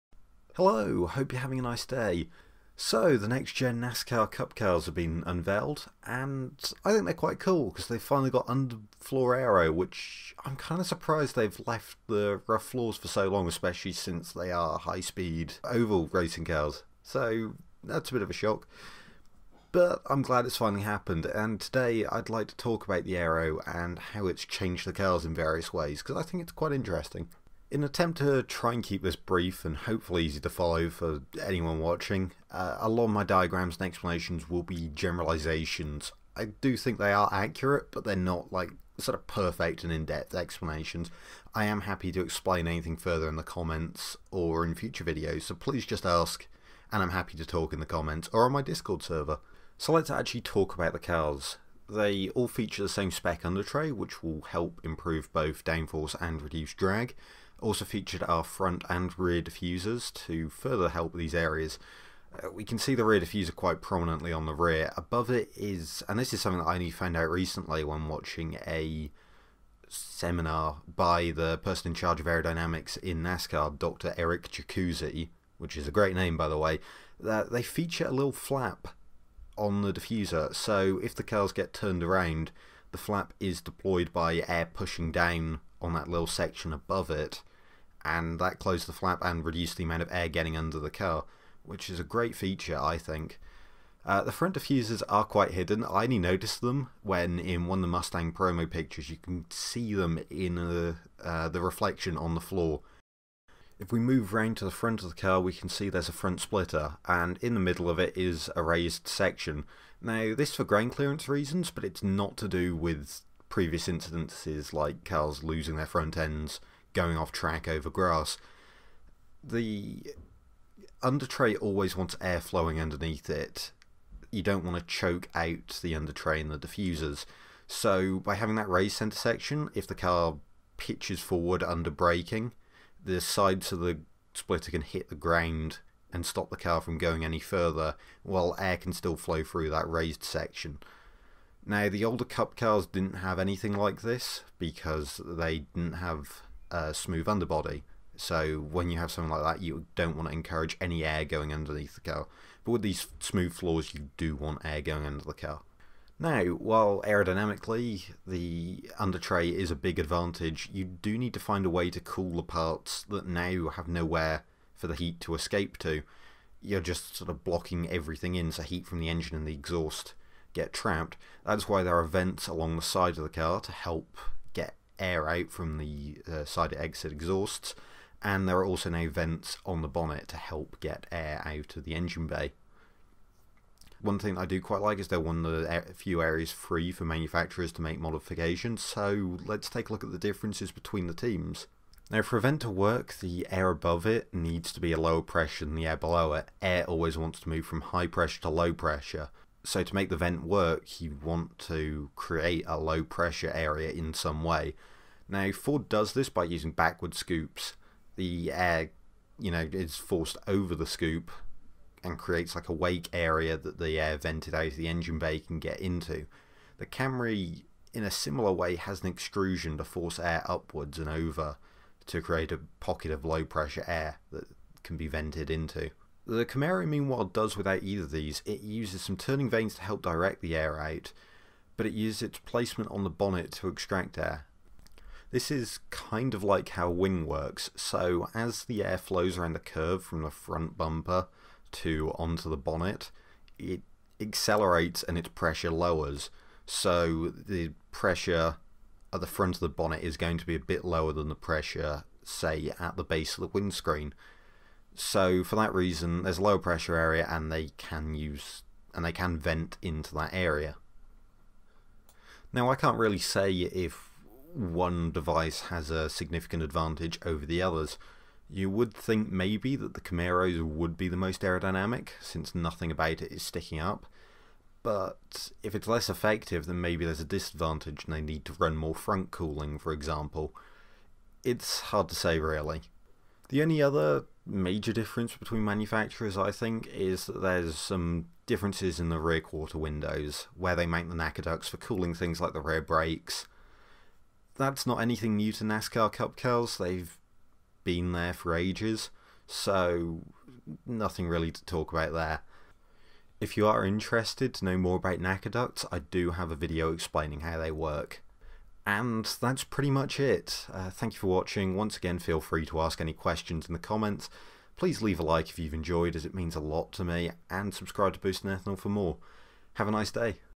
Next Gen NASCAR Aero Analysis
Today I give a few takes on the Next Gen package for the new NASCAR rules.